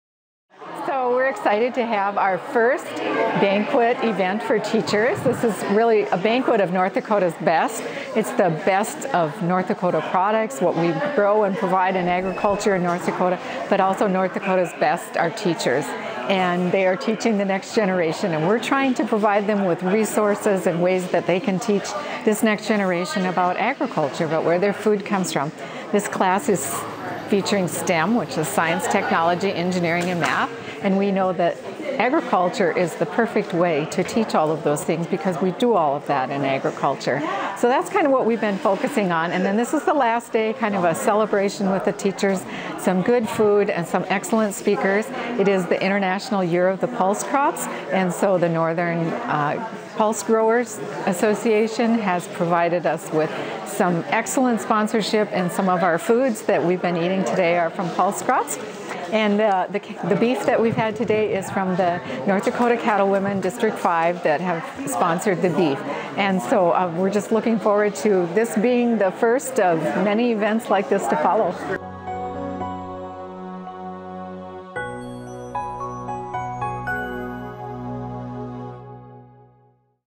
Teachers learn about ag at farm luncheon
Educators attending the Food, Land and People workshop in Bismarck, ND had an opportunity to enjoy a meal out at Menoken Farm to taste the goodness of North Dakota agriculture.